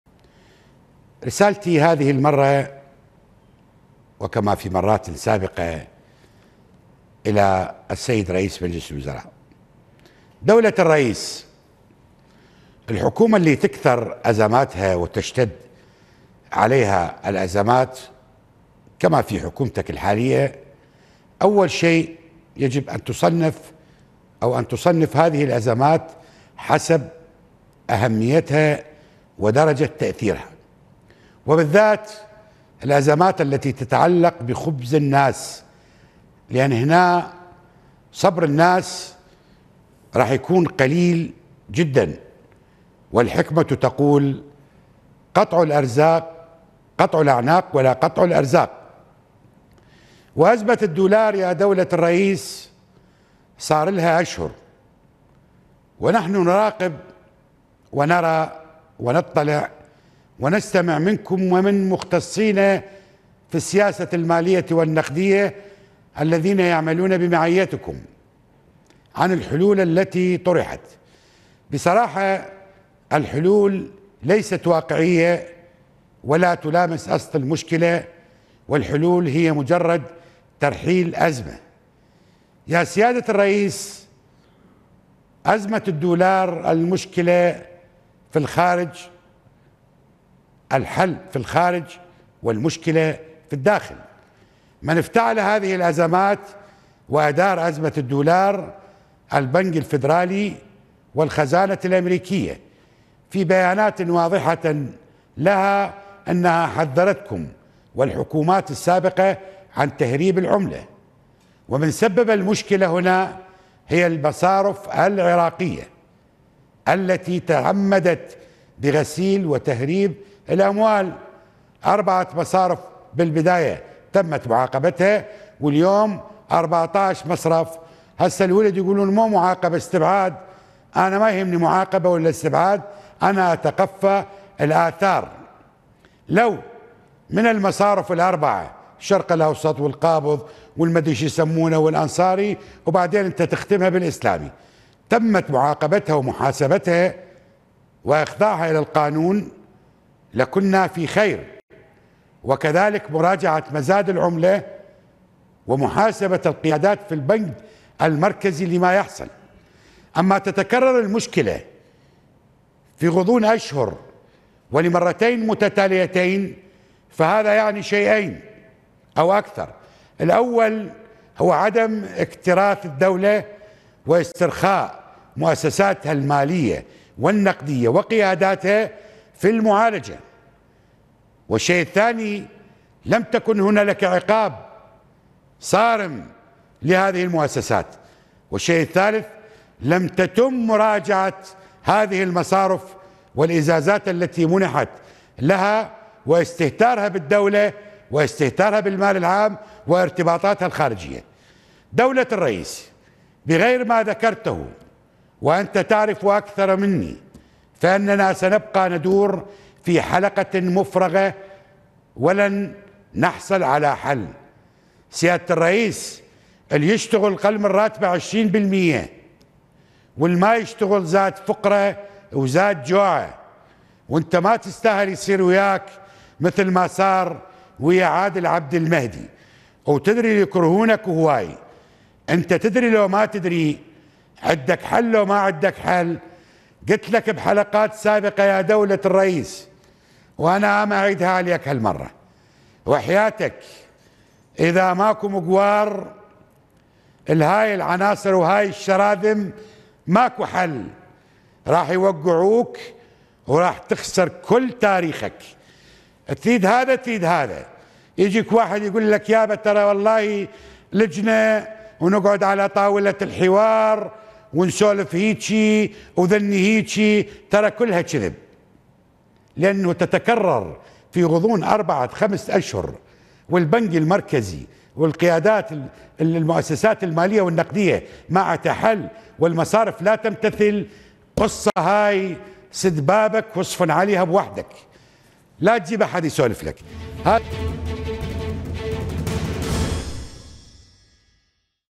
نجم الربيعي يحذر السوداني من هذا الامر
نجم الربيعي للسوداني: "احذر ان تخسر تاريخك"

موقع قناة التغيير الفضائية  الالكتروني